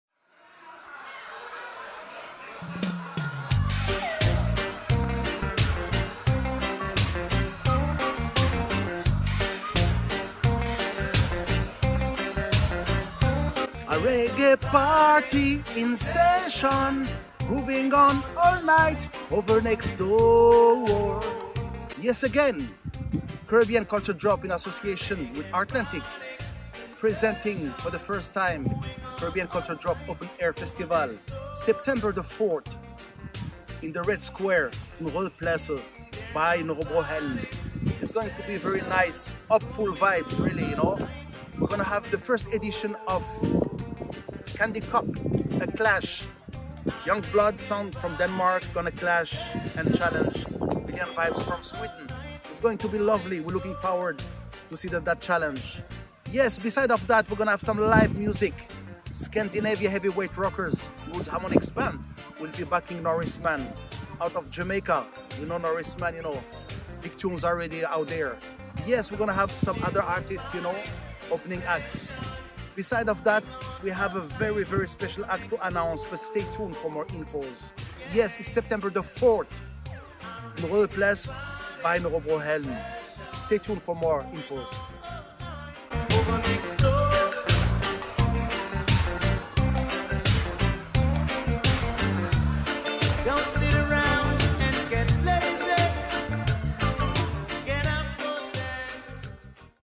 CARIBBEAN CULTURE DROP OPEN AIR FESTIVAL
ONE DAY REGGAE FESTIVAL, SOUND CLASH, LIVE PERFORMANCES, CARIBBEAN YARD FOOD AND DRINKS WILL BE AVAILABLE AT THE EVENT.